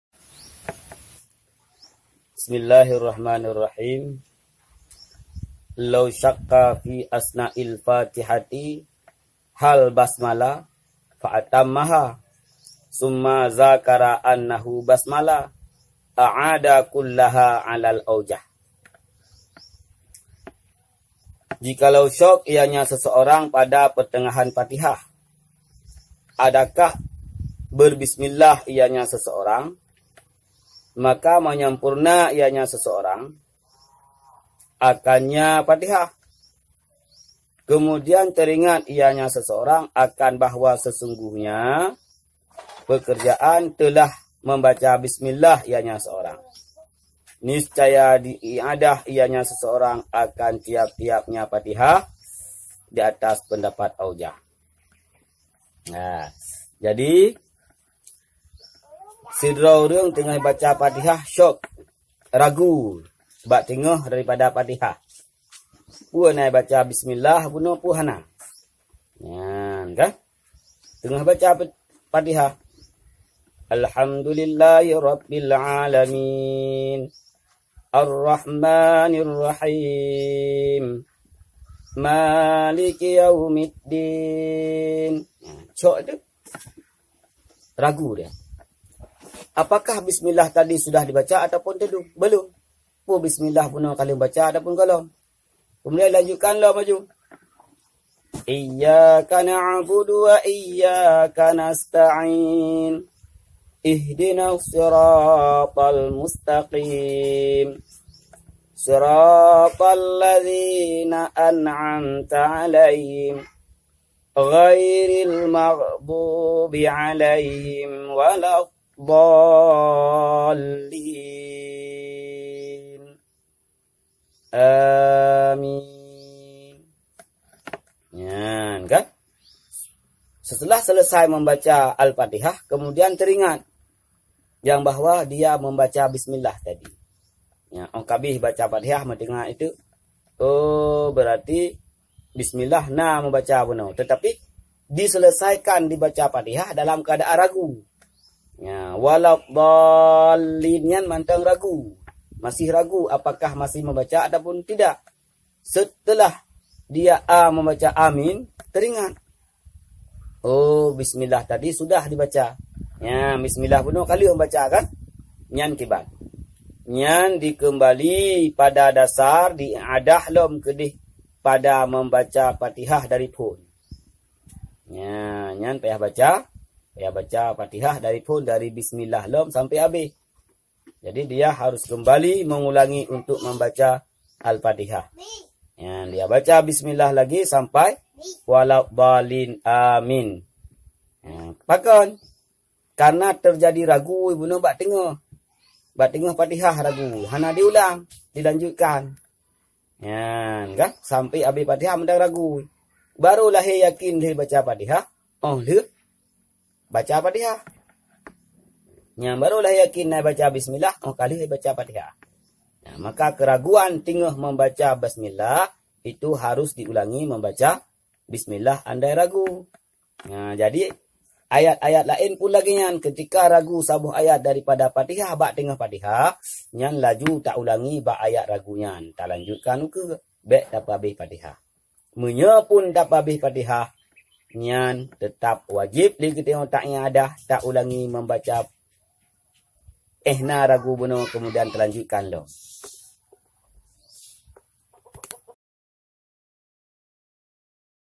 Ragu Pada Bacaan Bismilah, Kemudian Langsung Menyempurnakan Fatihah
Bolehkah Mneyelesaikan Bacaan Fatihah Sedang Ragu Apa Telah Membaca Bismilah?